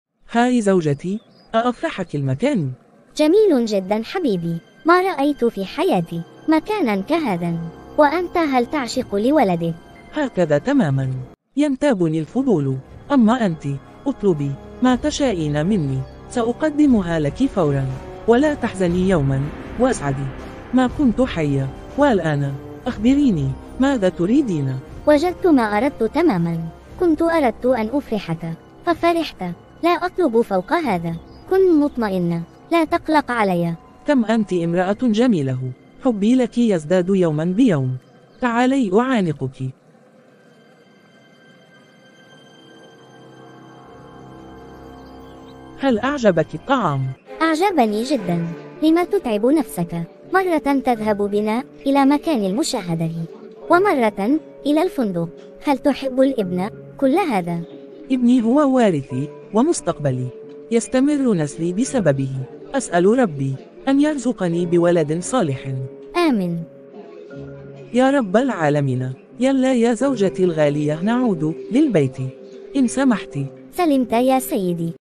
الزوجوالزوجةالحلقه[1][هاي زوجتي أافرحكِ المكان ؟هل أعجبكِ الطعام.زمن،2023
هذا الفيديو يتكلم عن حب الأب والأم لأبنائهم لأن الحب للأبناء أمر جميل جدا الإبن مستقبل الأب والأم الاِبن بسببه يستمر النسل والابن له فائدة كبيرة  فرح الأب لأبنائه كبير والابن يحيى طريق أبيه